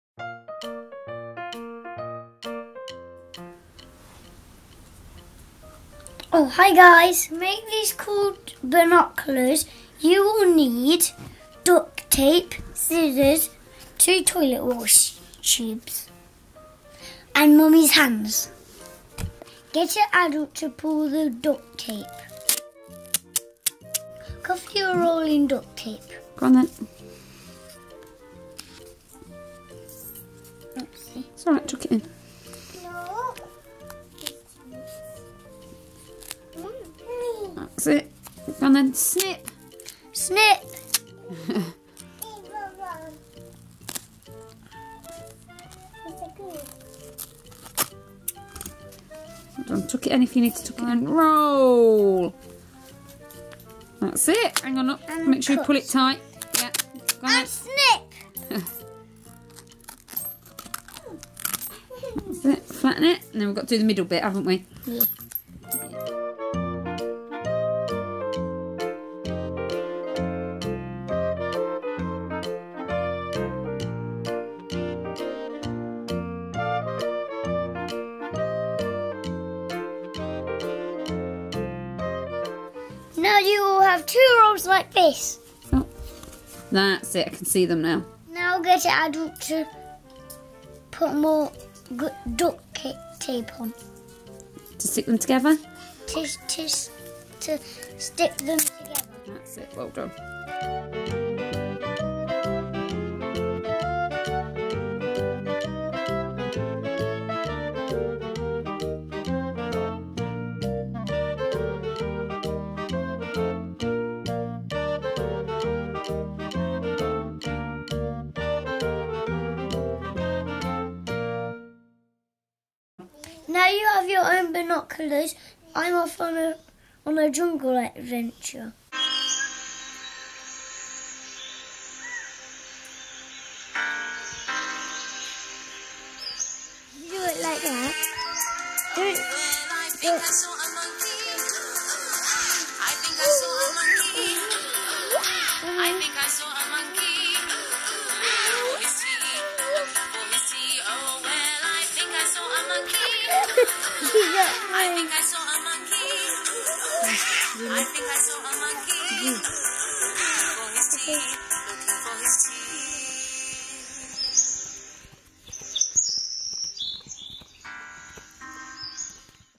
Duct Tape and Toilet Roll Tube Binoculars. Make It!
Jenson, 5, shows you how to make these easy to make binoculars using duct tape and toilet roll tubes.

To access EXCLUSIVE SECRET weekly videos join our FREE Members Only Kids Club